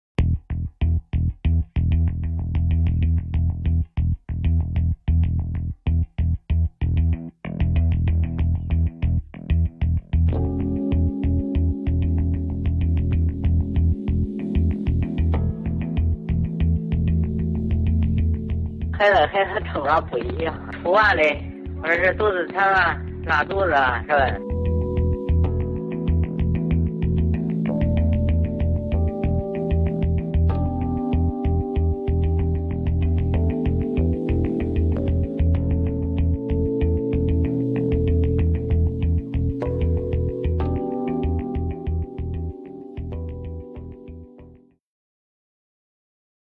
河北宁晋一所中学多名学生疑食物中毒 就诊学生有腹泻等症状
9月21日，河北邢台宁晋十三中多名学生出现疑似食物中毒症状。有家长反映，学生吃了学校饭菜后出现呕吐腹泻，目前已送医。宁晋妇幼保健院工作人员称，该院目前接收到大概7名学生，有学生出现腹泻等症状。上述工作人员还表示，除宁晋县妇幼保健院外，宁晋县其他医院也有接收到该校就诊学生。记者致电宁晋十三中学校长，对方称学校正在处理。宁晋县教育局和市监局工作人员表示此事正在处理中。